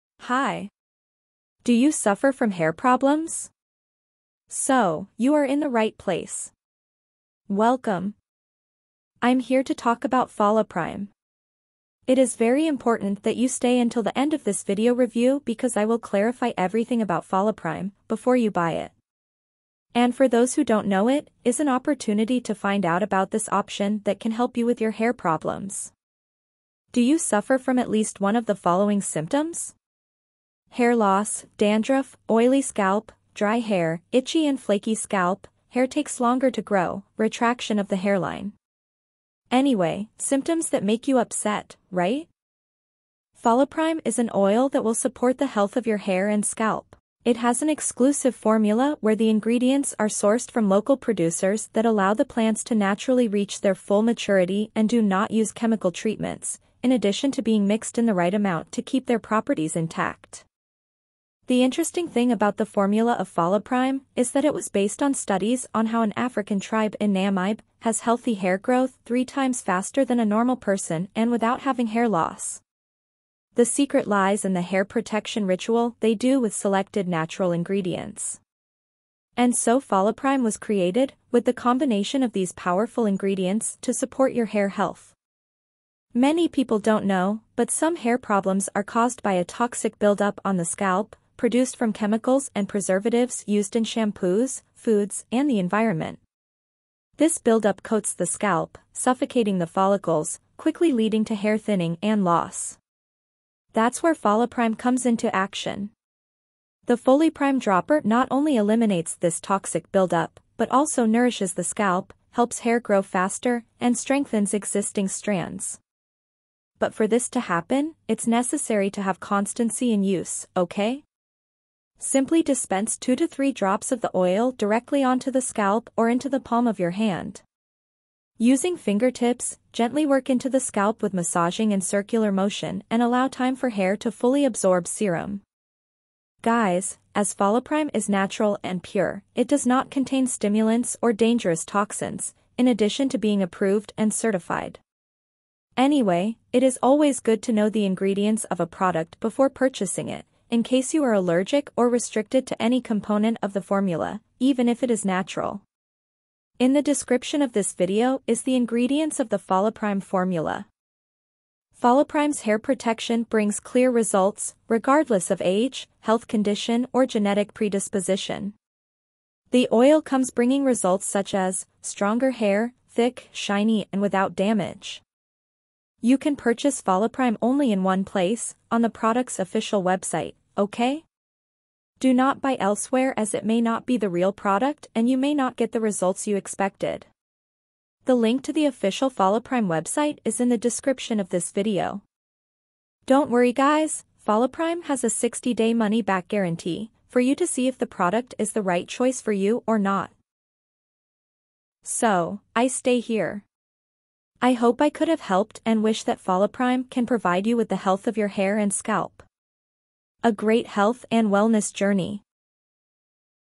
FoliPrime Review (⚠️ Know The Truth ⚠️) - Hair Support Formula
🔹 What does FoliPrime do?
Foliprime is an oil with natural ingredients that supports the health of your hair and scalp.

🔹 How does FoliPrime work?
FoliPrime eliminates toxic buildup on the scalp, nourishes, helps hair grow faster and strengthens existing strands.

🔹 What are the ingredients in FoliPrime?
Inside FoliPrime you’ll find: Lemon Peel, Castor oil, Niacin, Biotin, Stinging Nettle, Hyaluronic acid, Cayenne pepper, Turmeric, Zinc Oxide, Argan oil, Tea tree, Olive oil and Almond oil.

🔹 Does FoliPrime have a guarantee?
Yes. FoliPrime has a 60-day money back guarantee.

🔹 Where can I buy FoliPrime?
FoliPrime is only sold on the official website.